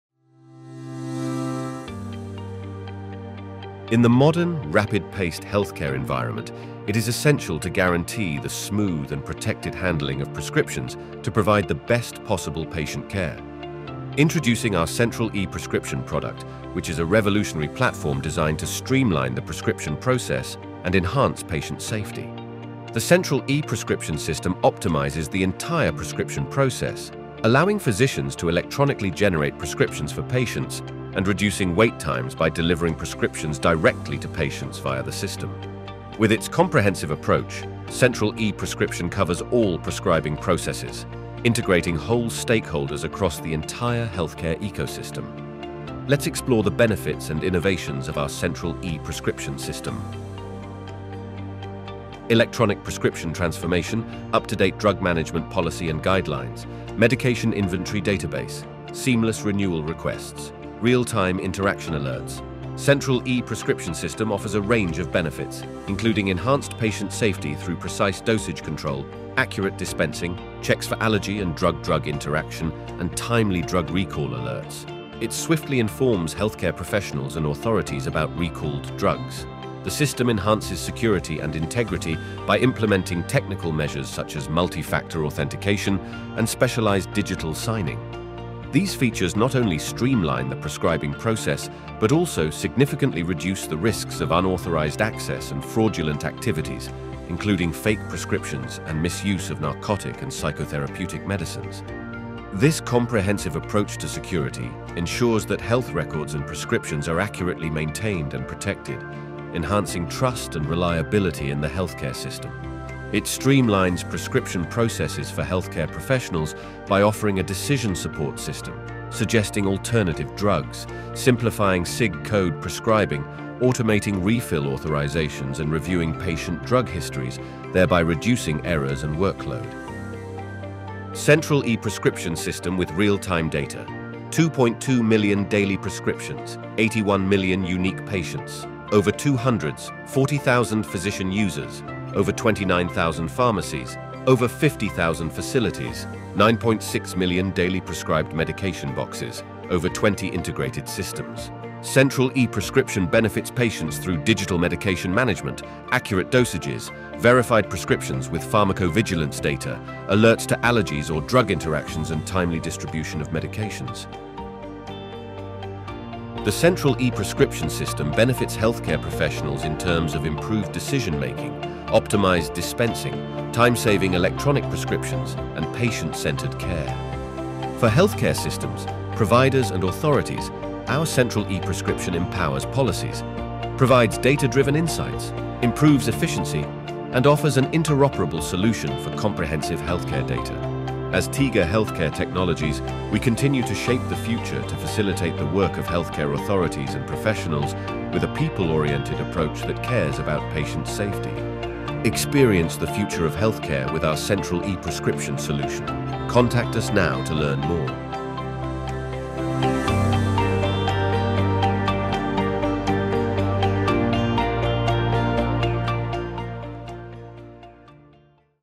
Revolutionizing Prescribing Process with Seamless Technology: Tiga’s Central e-Prescription
Welcome to Tiga Healthcare Technologies, where we transform healthcare delivery through innovative IT solutions. Join us as we delve into the world of the Central e-Prescription System and electronic prescription transformation's indispensable role in safe and connected prescription management.

Central e-Prescription revolutionizes prescribing procedures by integrating all stakeholders in a country. This comprehensive framework facilitates seamless communication between pharmacies and healthcare providers for efficient and secure management.

Centralizing and digitalizing these operations and optimizing the prescribing journey transforms the healthcare IT environment. Furthermore, this system supports the real-time tracking of prescriptions and patient adherence. Eventually, it enhances the quality and efficacy of patient care.

We shape the future of healthcare with our national-scale Central e-Prescription System, which over 246,000 physicians have already trusted to prescribe medications to 81 million patients.  Let's redefine what's possible in safe and patient-centered healthcare IT innovations. Let's shape the future together, as always!